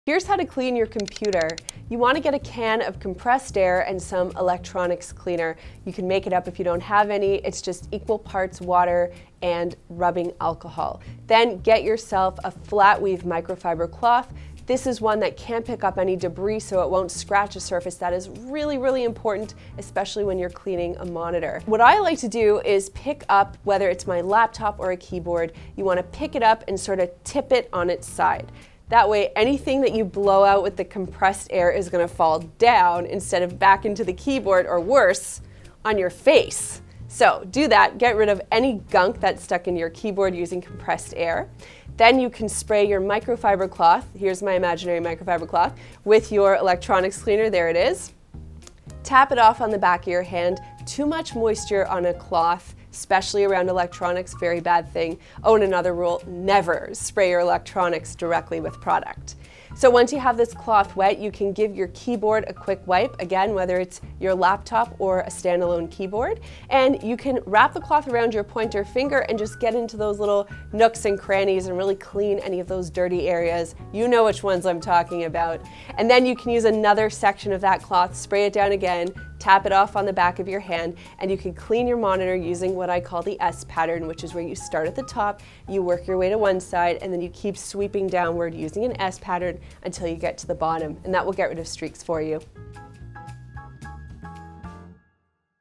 How to clean your computer or laptop safely
Cleaning expert and author of "Clean My Space" Melissa Maker breaks down how to clean your computer or laptop without damaging it